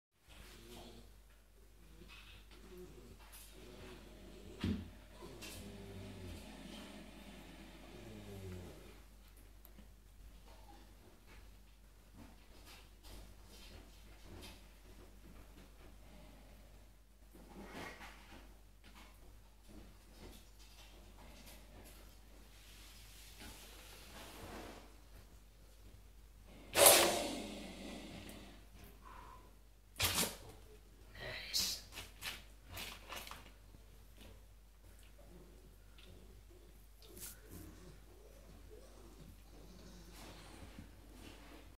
bosc monitor whip
Our wee Hulk showing how quick he can move when he wants to.